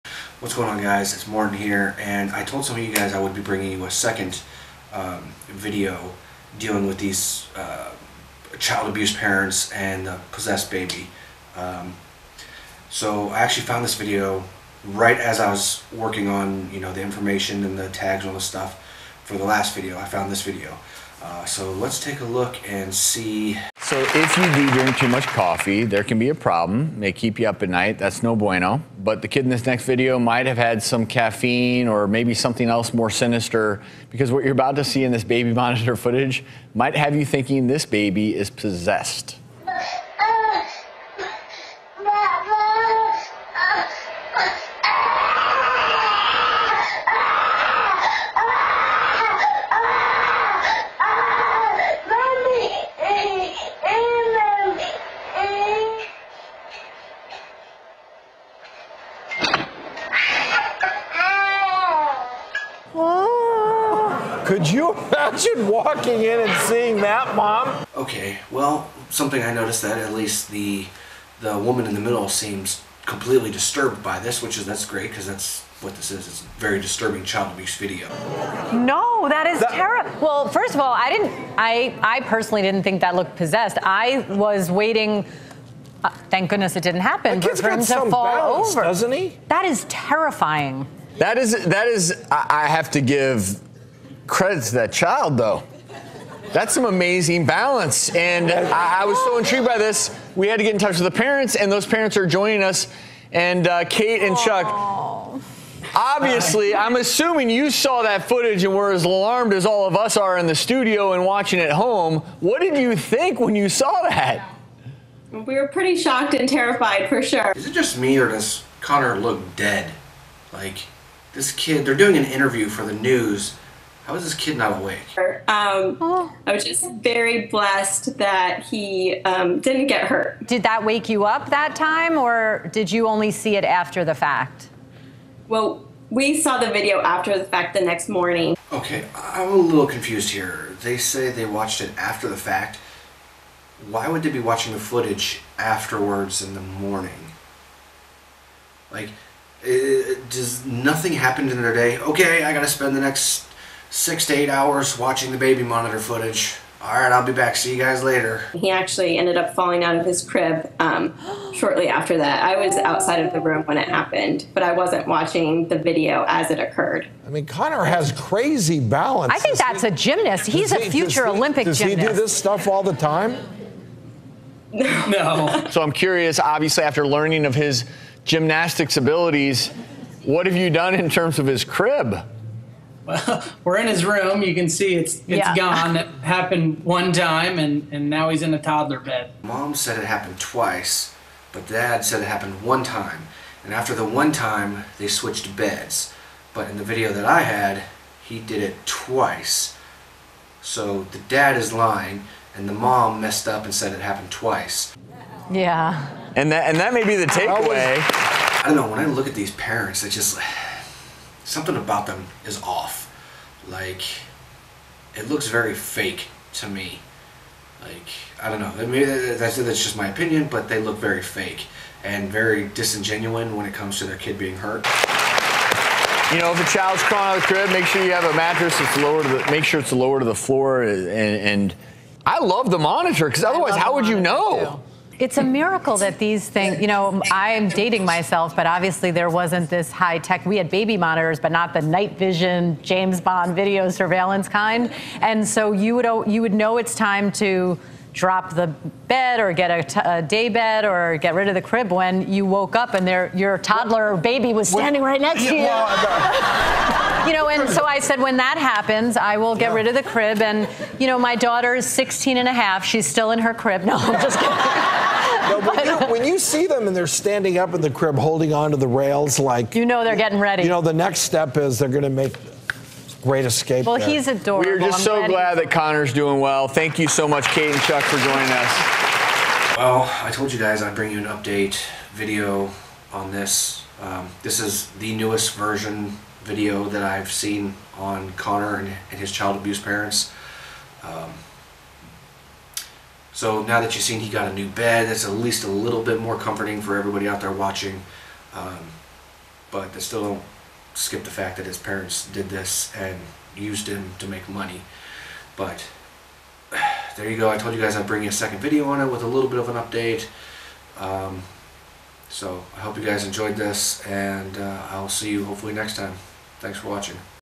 Possessed Baby Crib Surfing Incredible Balance Exposed Pt2
DDFGhostHunting merchandise!

Many you asked for a second part. I figured, I would show you more into the lives of these scumbag child abusing people. Watch as they lie on tv an take the whole child abuse thing as a joke.